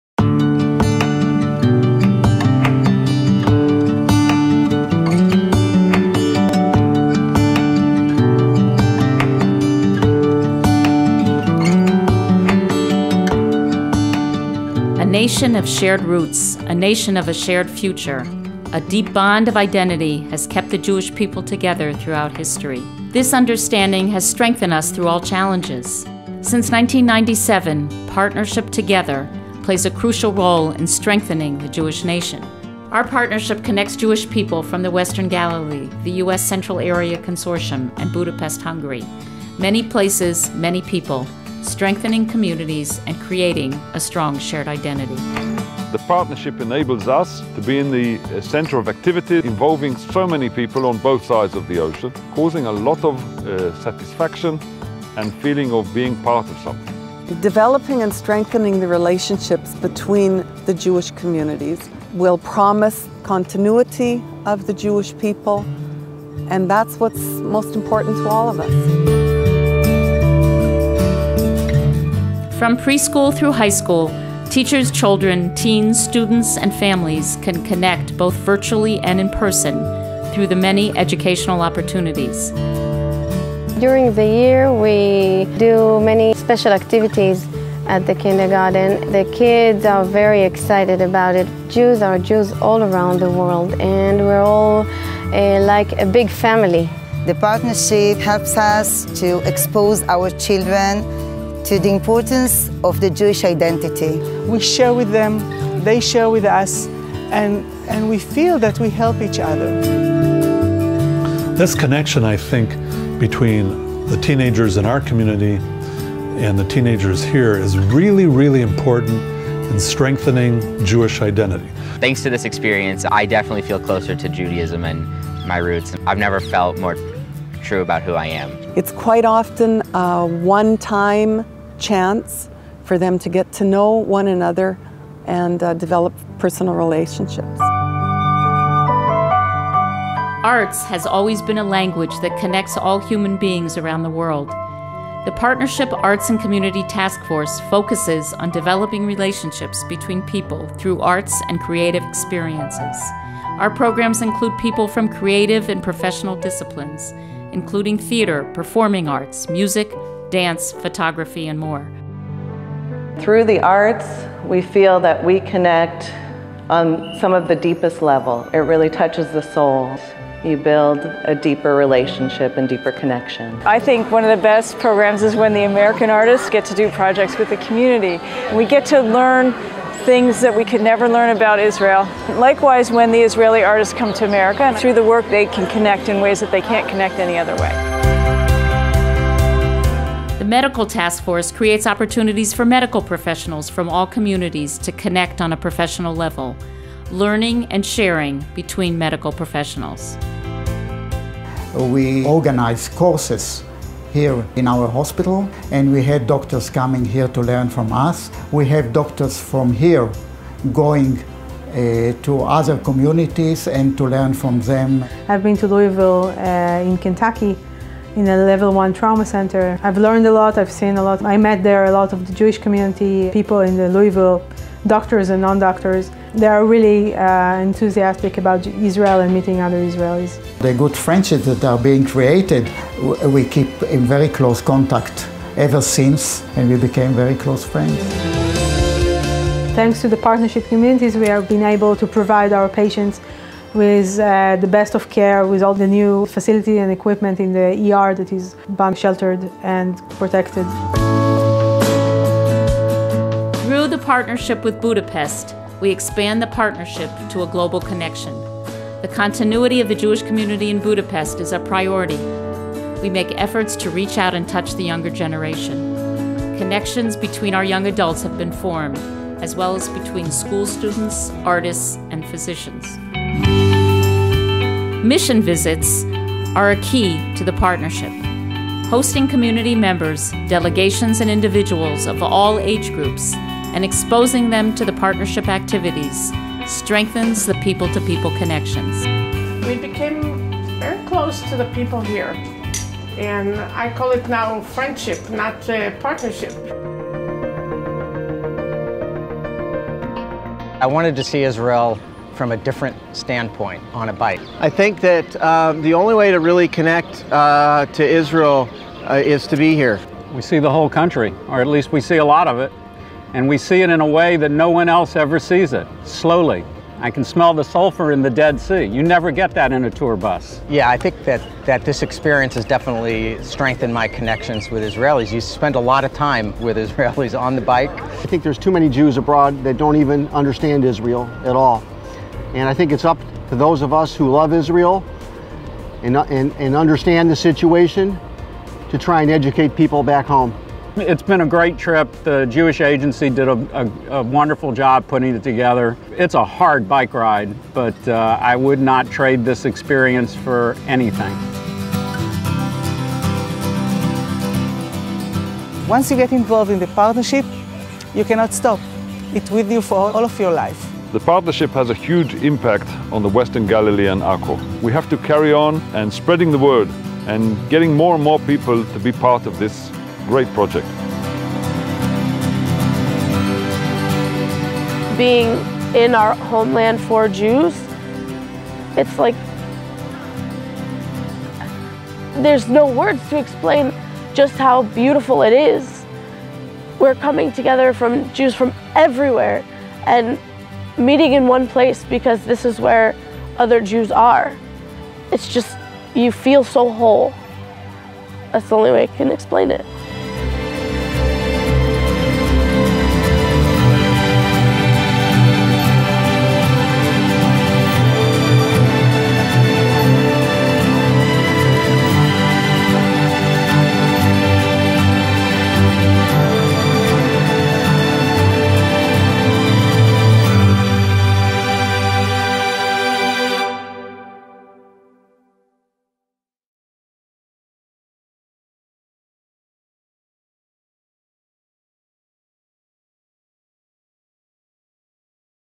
סרט תדמית לשותפות ביחד - גליל מערבי
הסרט מתעד את הפעילות של השותפות והפרויקטים הרבים שמקדמים חיזוק הקשר עם יהודי התפוצות וחיזוק איזור הגליל המערבי